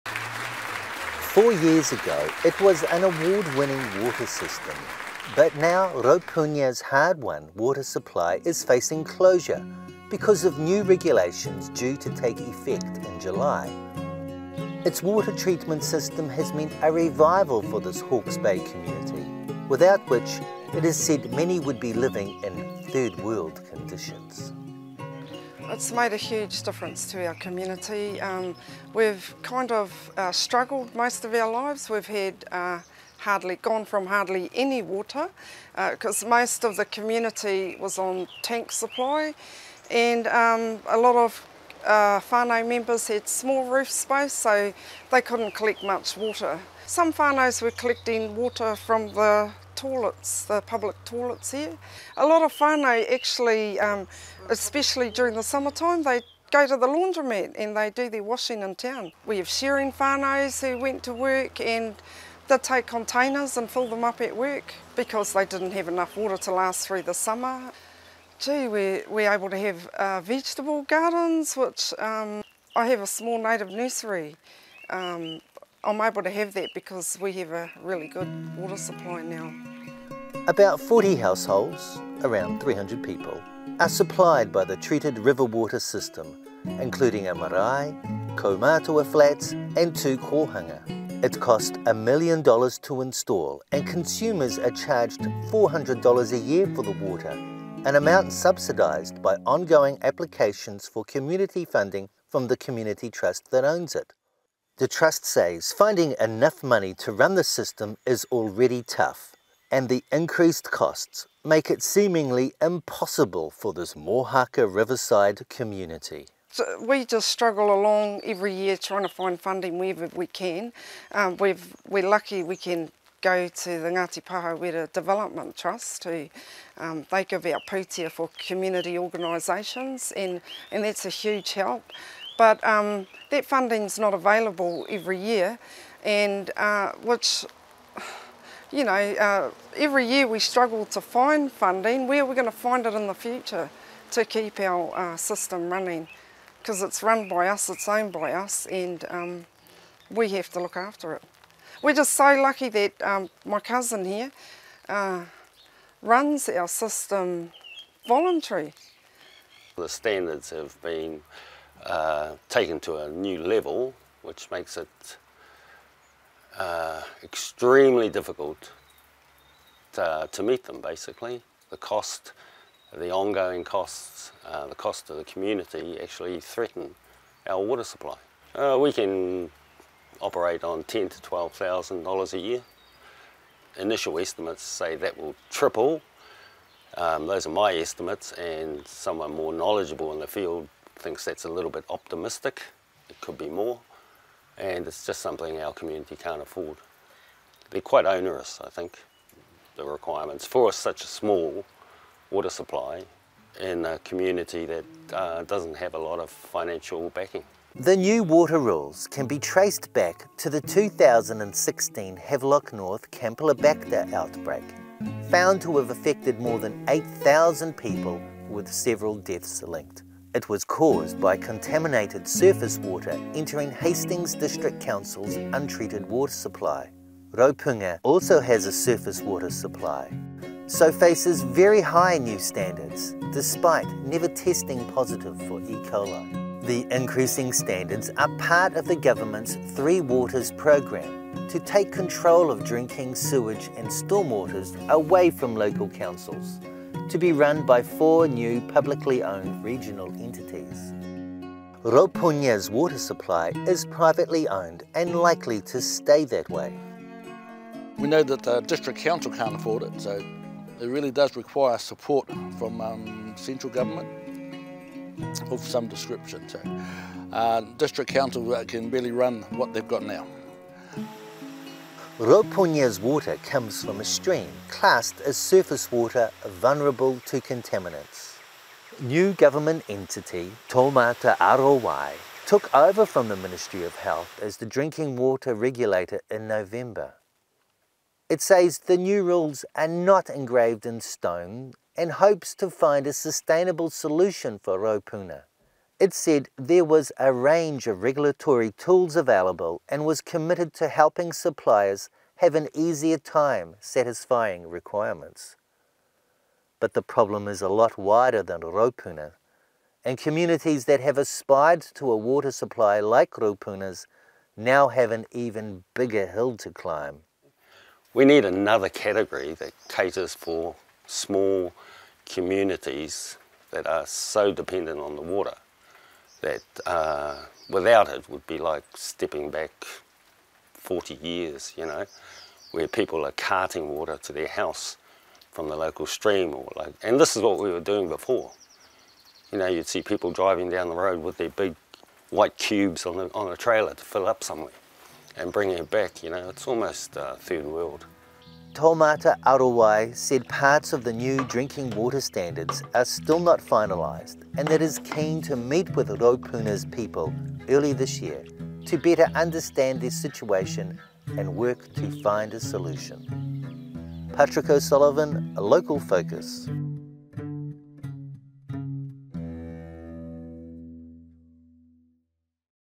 Hawke's Bay community fears for water supply under new rules | Local Focus
Raupunga's water system won an award from the Hawke's Bay District Health Board four years ago, but under new regulations due in July it may have to close it.

About 40 households - about 300 people - are supplied by the treated river-water system, including a marae, kaumātua flats and two kōhanga.

Public Interest Journalism funded through NZ On Air.